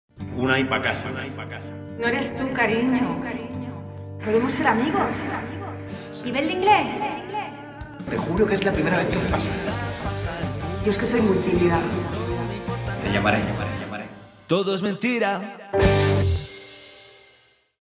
TODO ES MENTIRA (Promo) - Teatro Fígaro
Más de 20 años después de su estreno en cines, con un elenco de lujo y gran vigencia para las nuevas generaciones. Todo es mentira, dirigida por Quino Falero, revive, esta vez en formato escénico, amor, humor, sexo, crisis, amistad, relaciones, convivencia... hay cosas que nunca pasan de moda.

Del 20 de octubre de 2016 al 8 de enero de 2017
De miércoles a viernes 20:00 h., sábados 18:30 h. y 20:30 h., Domingos 18:00 horas.
​Funciones adicionales los lunes: 24 octubre, 26 diciembre y 2 enero.
Sábado 22 de octubre solo una función a las 20:00 horas.